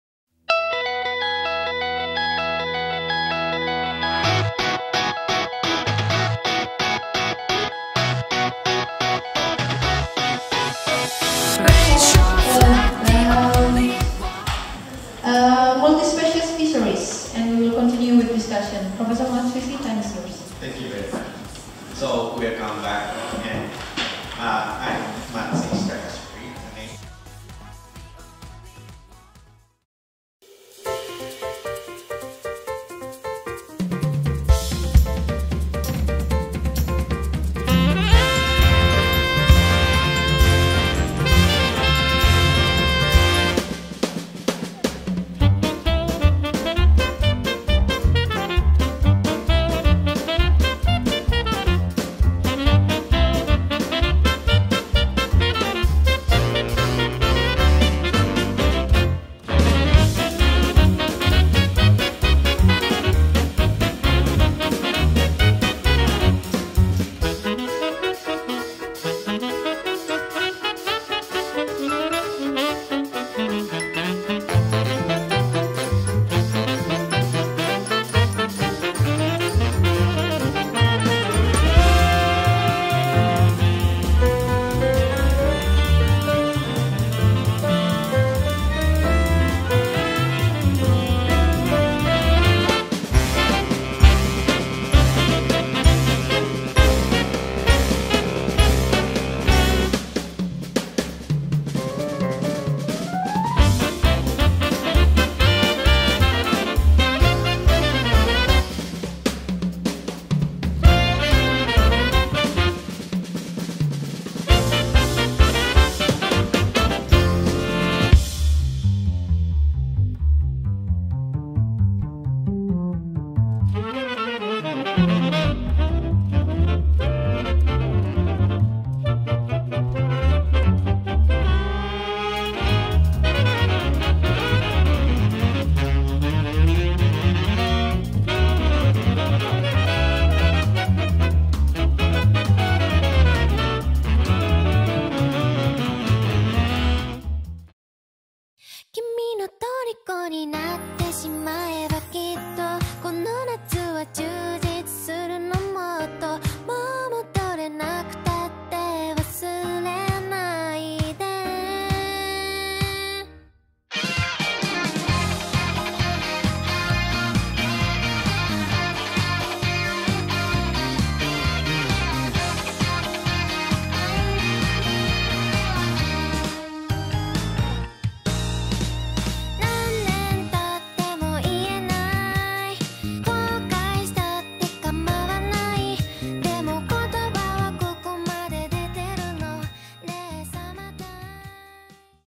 Short Training on Biodiversity and Multispecies Fisheries (Learning Satellite 2023)
A collaboration event between Universitas Brawijaya and Hokkaido University which held "Short Training on Biodiversity and Multispecies Fisheries". Located in the Main Building of FFMS UB and Marine Station Sendang Biru, this activity took place on 31 July - 4 August 2023 with 5 students of Hokkaido University, 9 students of UB, 2 students of Universiti Malaysia Trengganu, 2 lecturers of Hokkaido University and 15 UB lecturers are participated in this activity.